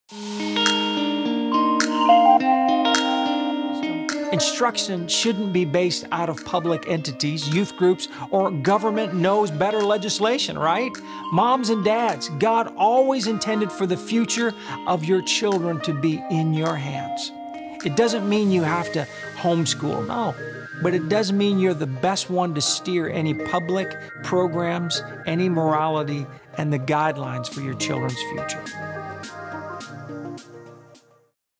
THEIR FUTURE IS IN YOUR HANDS // Pastor Stephen Marshall
Keys of help to guide your children to a fruitful future.

We are here to cover, value & encourage you to LIVE LIFE STRONG! For inspiration and answers go to: